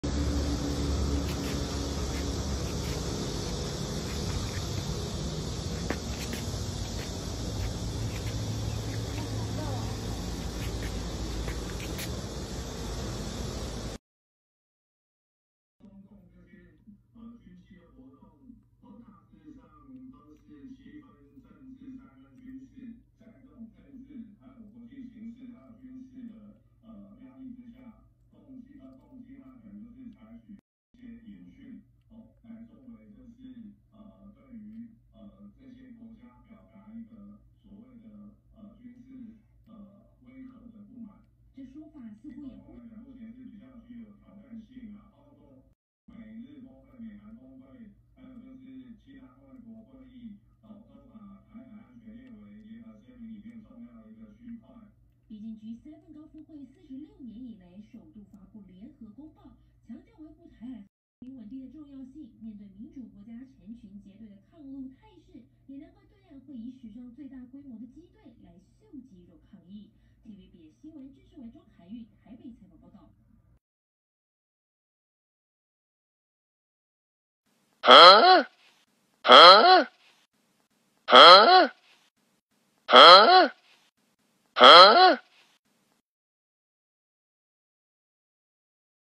入手蘇卡達!!! 曬太陽很重要!!! take a walk with sulcata turtle and dinner time
Let’s Turtle